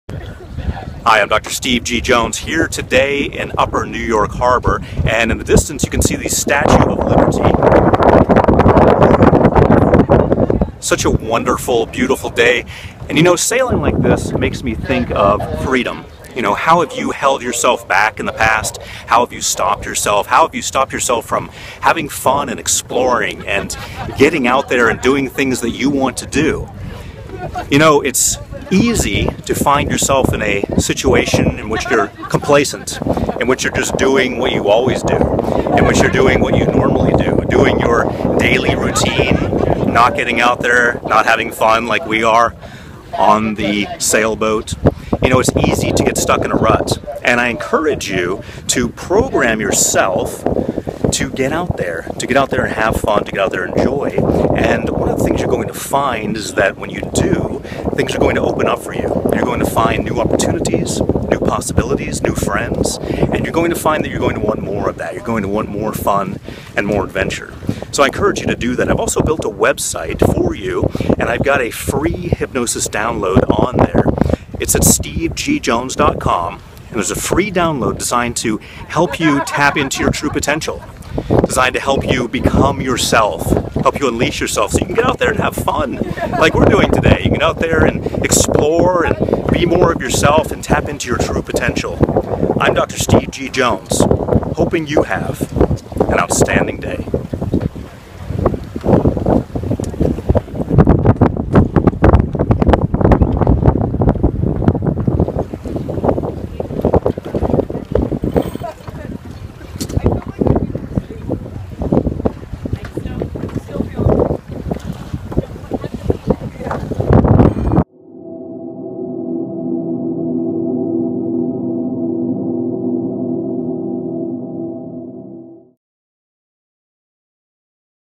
Be Adventurous (1 of 2) - Dr. Steve G. Jones
For more information about hypnosis and discovering your true potential, please

Dr. Steve G. Jones, clinical hypnotherapist, in Upper New York Harbor, sailing past the Statue of Liberty, talking about the importance of being adventurous, reaching out and expanding your horizons.